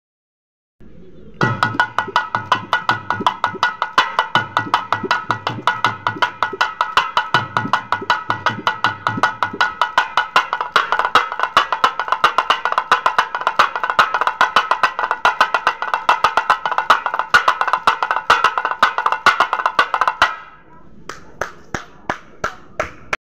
Thavil Dr. Abdul Halim
Dr. Abdul Halim played thavil in this video